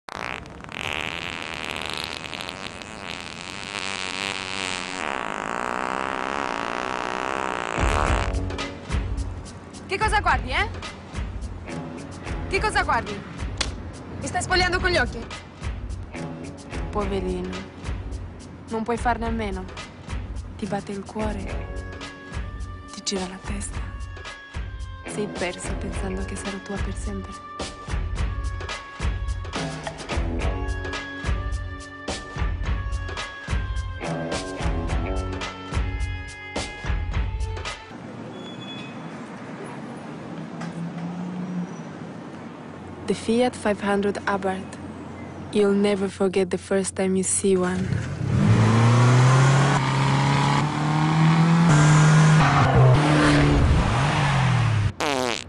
Fiat Farts
I put some fart sounds in this Fiat commercial